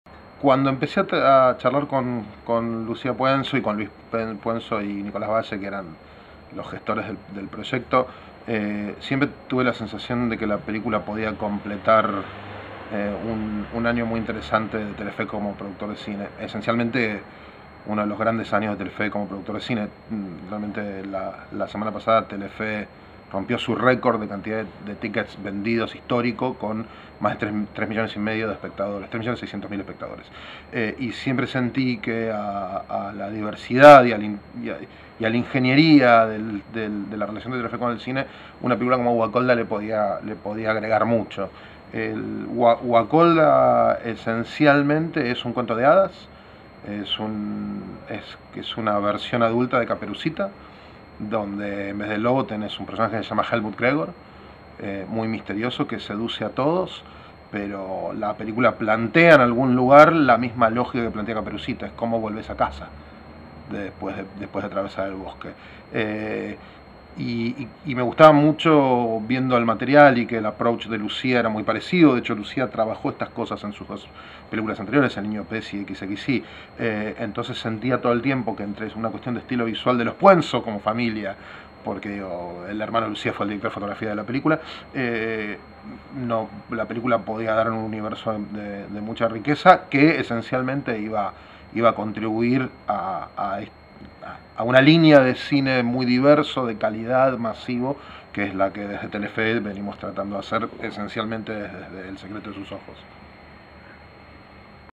Wakolda por Axel Kuschevatzky: "Wakolda es una versión adulta de caperucita"
WAKOLDA
una película de Lucía Puenzo
Con Alex Brendemühl, Natalia Oreiro, Diego Peretti,
Elena Roger, Guillermo Pfening, Ana Pauls, Alan Daicz
y la presentación de Florencia Bado.